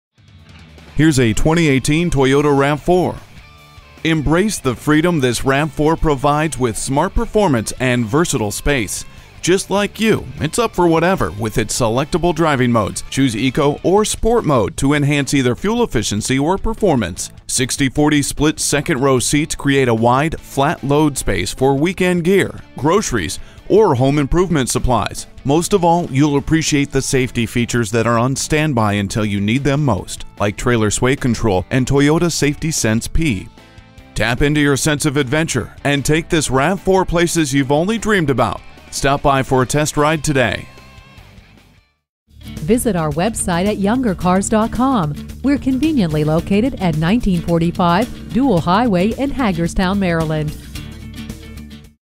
New 2018 Toyota RAV4 Frederick MD Hagerstown, WV V2758300 - SOLD
Year: 2018
Make: Toyota
Model: RAV4
Trim: SE
Engine: 4 Cyl. 2.5 L
Transmission: Automatic
Color: Magnetic Gray Metallic
Stock #: V2758300
VIN: JTMJFREV9JD235891
The 2018 Toyota RAV4 deliver more fun with a powerful 176hp 4-cylinder engine for off-the-line thrills. Coupled with the Dual Variable Valve Timing with intelligence technology, the RAV4 is a smart, spirited choice. More room means more space for gear and friends. You can quickly reconfigure the rear seats into a 60/40 split and fold the second-row seats down to create a wide, easily accessible load space with up to 73.4 cubic feet of capacity. RAV4 offers two available audio systems that incorporate navigations, so you can have the perfect soundtrack for all of your adventures. Travel safely with more peace of mind. RAV4 comes standard with a suite of six advanced safety features designed to help keep you out of harma€TMs way.

Be One!

Address:
1945 Dual Highway